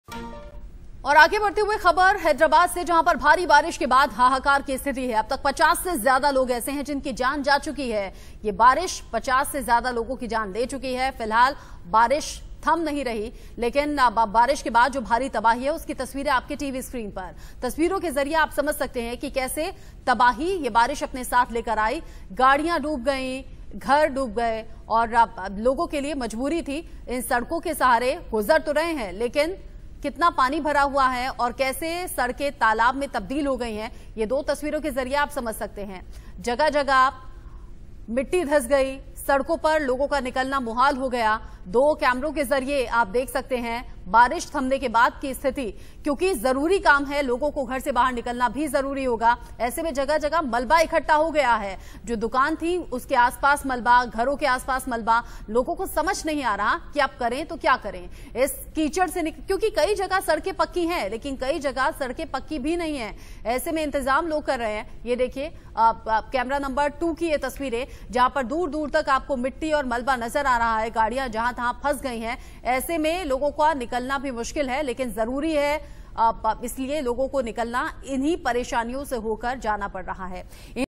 Hyderabad Rain: हैदराबाद में भारी बारिश के कारण मौत का आंकड़ा पहुंचा 50 तक | India News
Inkhabar (India News), the leading news channel for news in Hindi in India. Get Latest News Video Updateds.